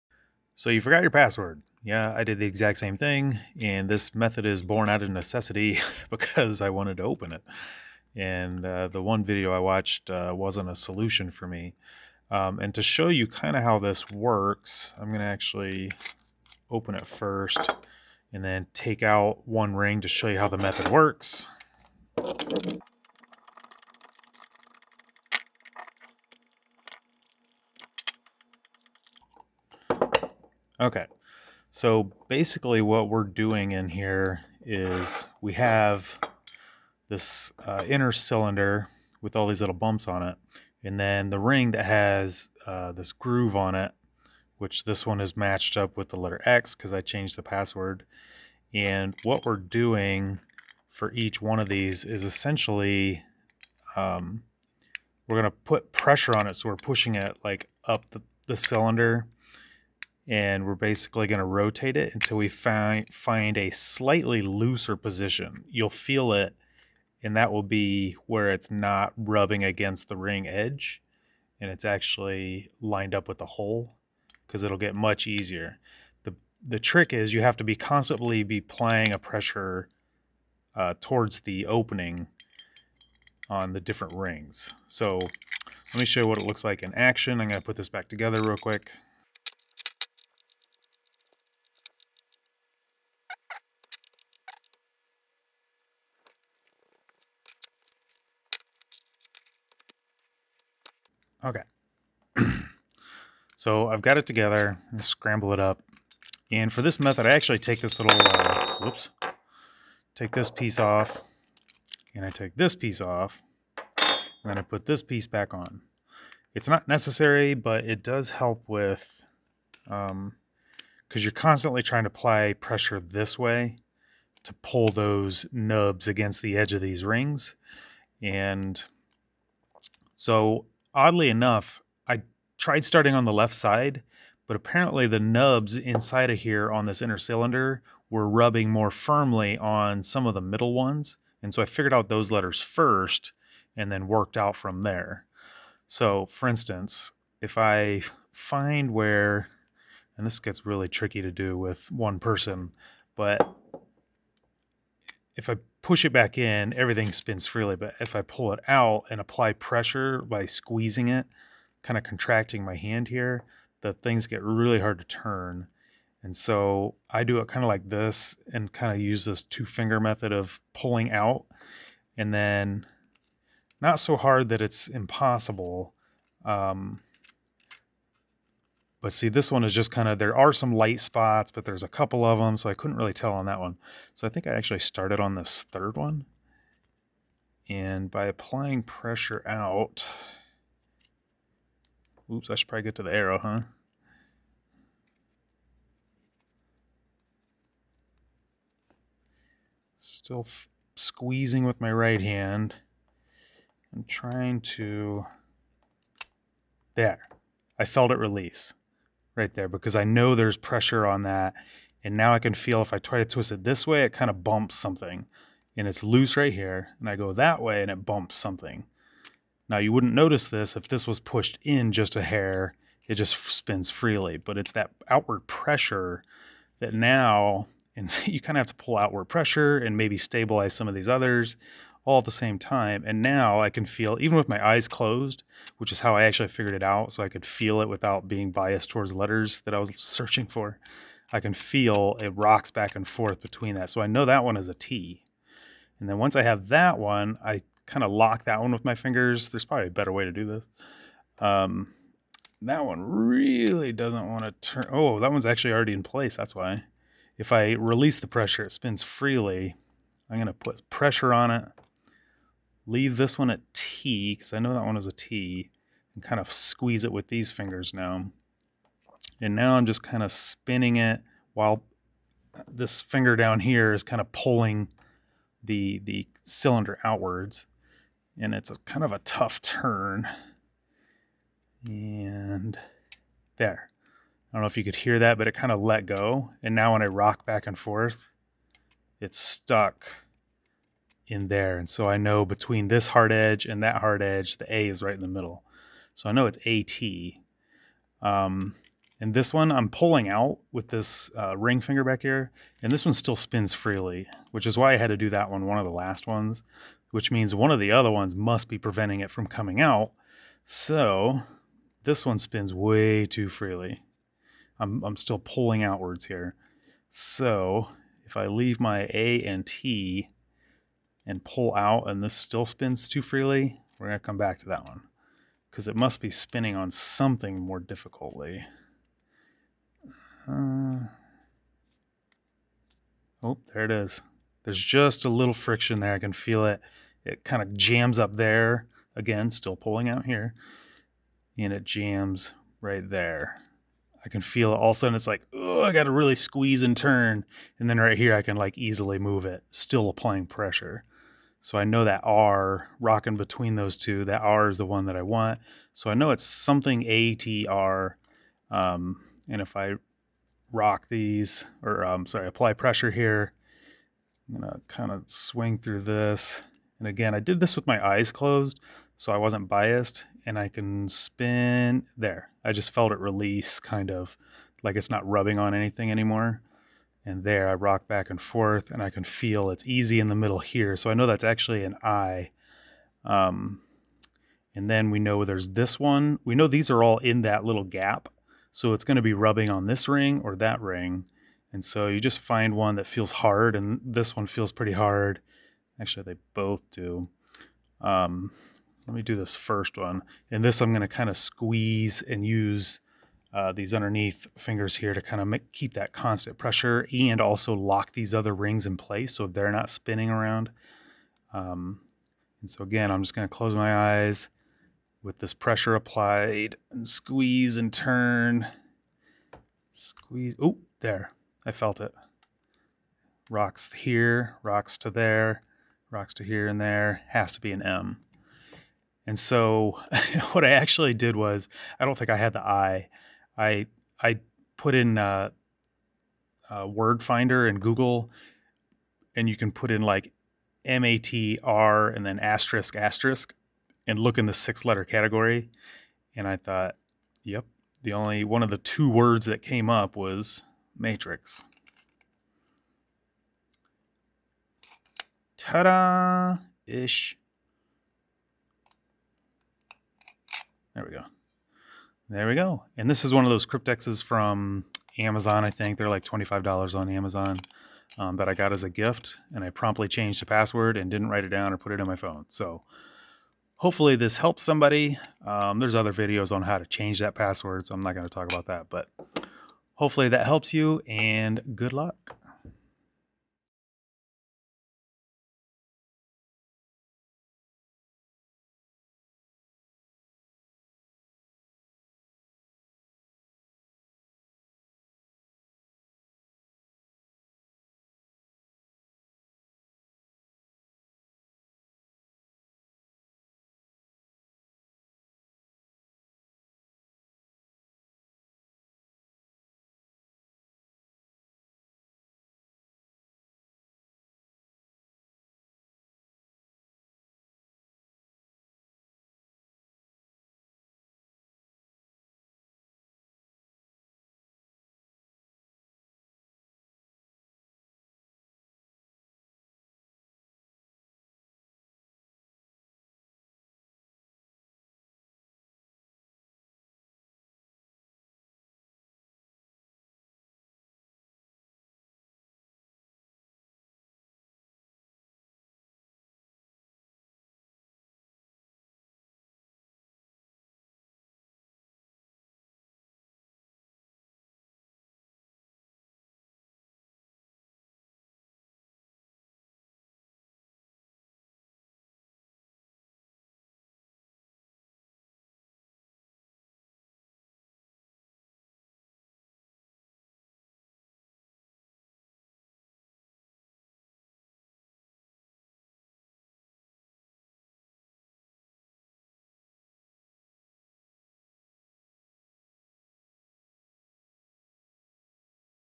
Hack and Break into a Cryptex
I forgot my password, but figured out a way to break in without damaging the cryptex!

FYI, Your hands will probably cramp up at some point doing this.y